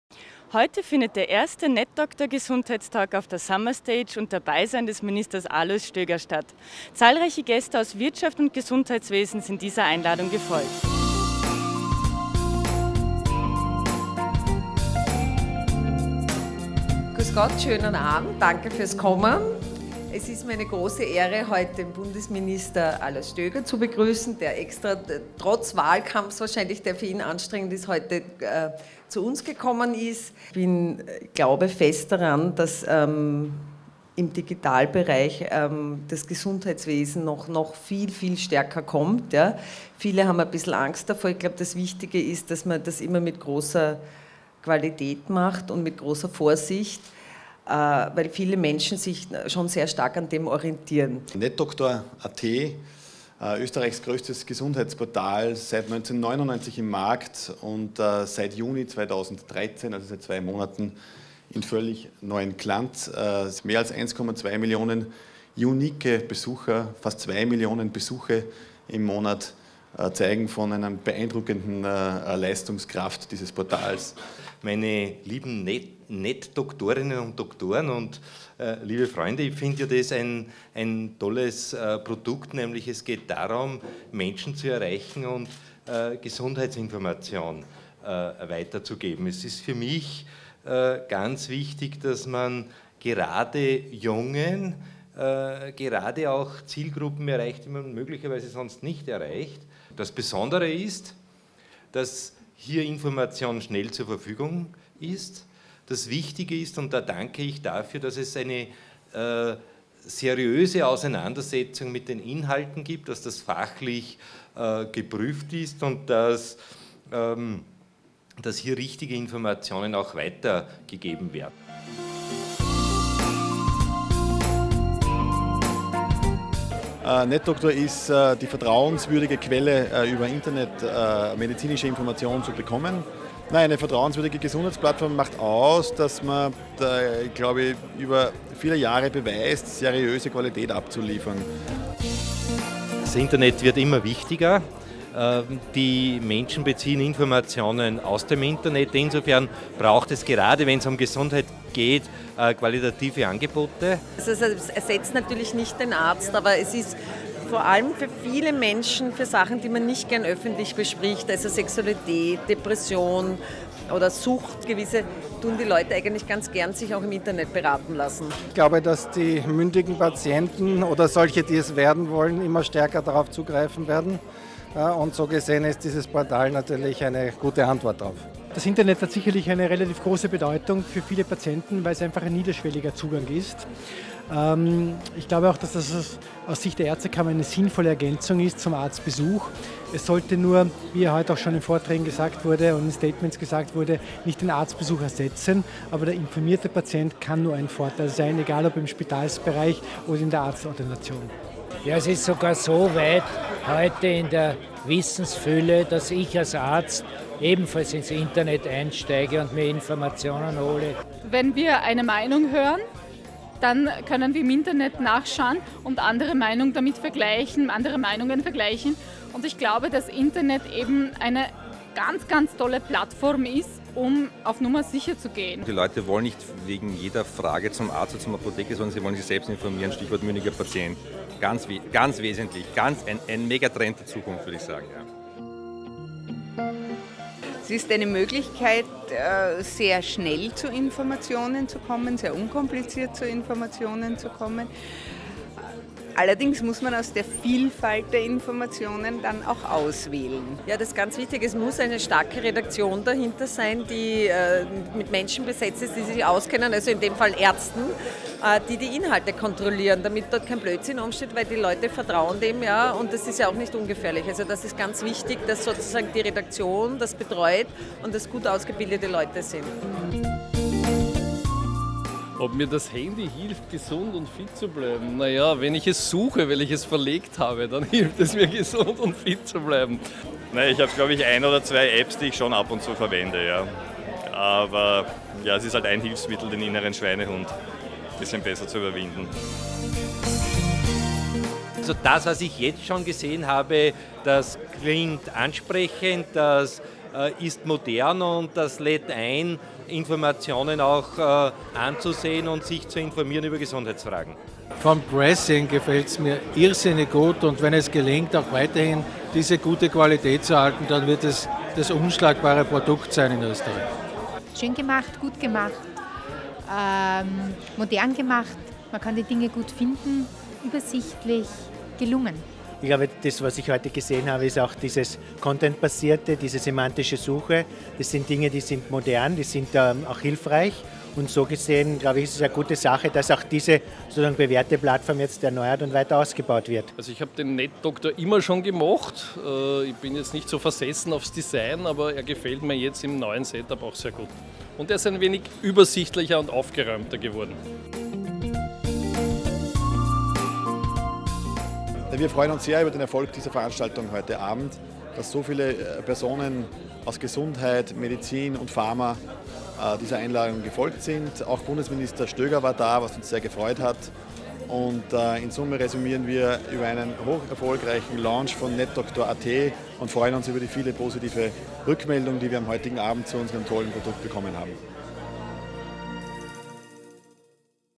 netdoktor.at Gesundheitstalk (2.9.2013)
Mehr als 50 hochkarätige Gäste waren zur Präsentation der neu gestalteten auf die Wiener Summerstage geladen.